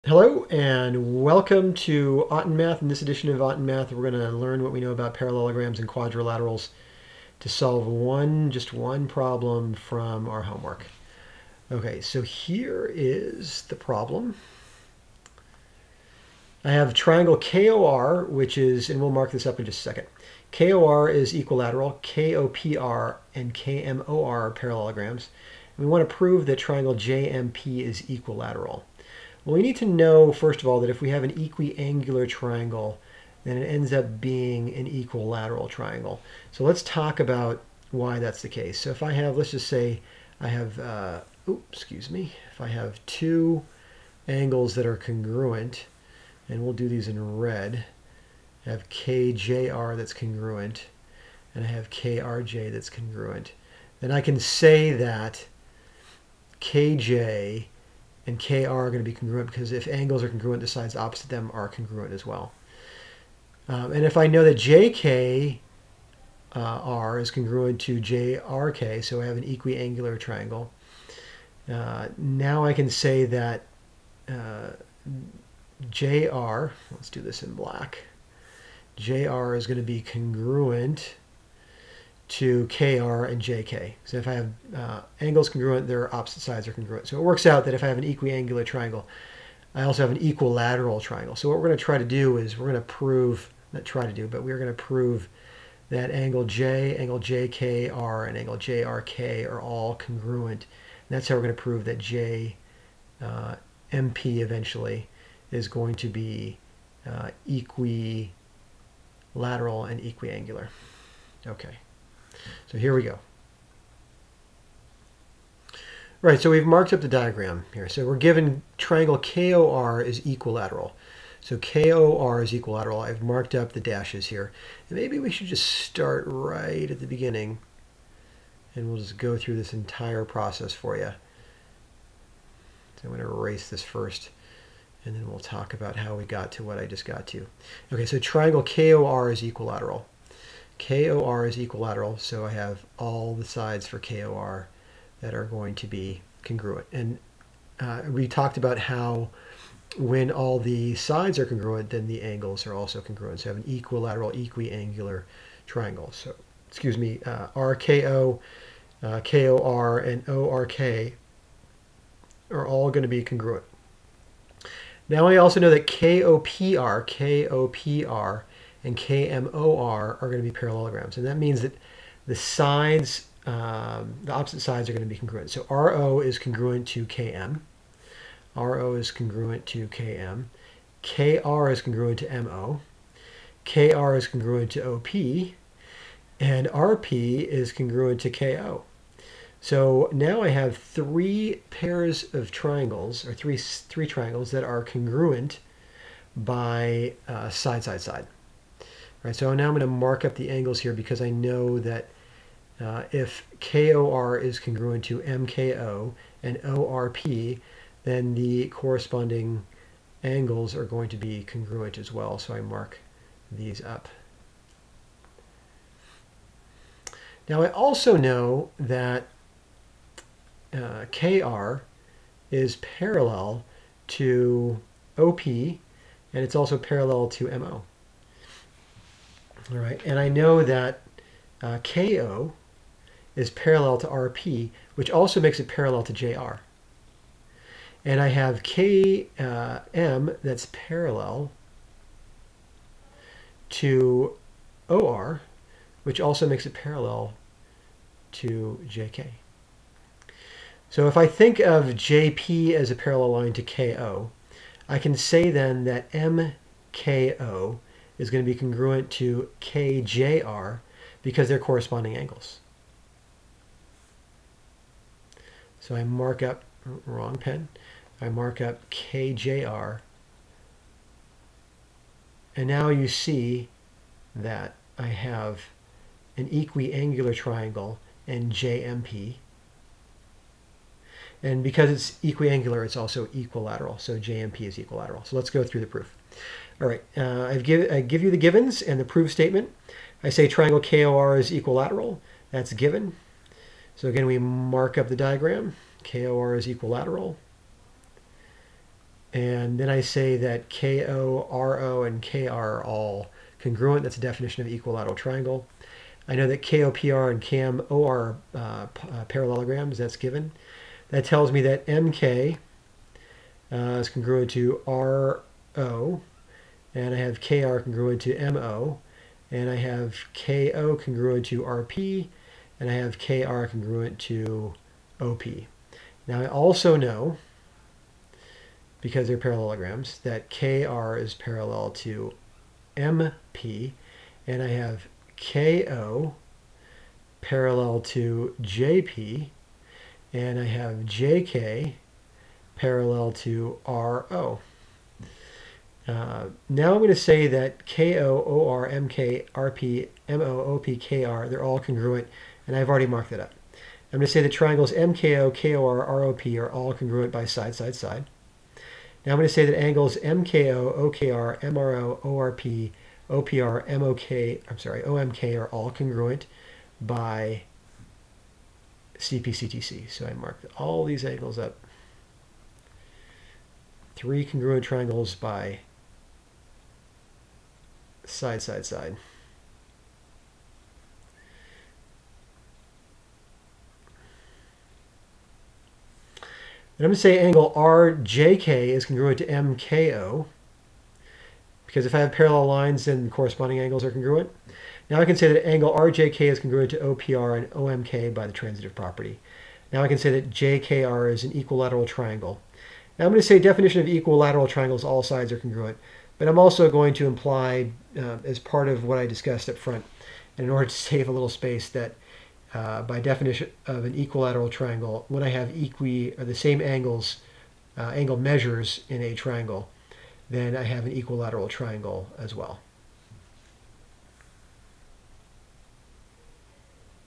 5.6 Proving that a Quadrilateral is a Parallelogram (Practice)
Practice problems involved in showing a quadrilateral is a parallelogram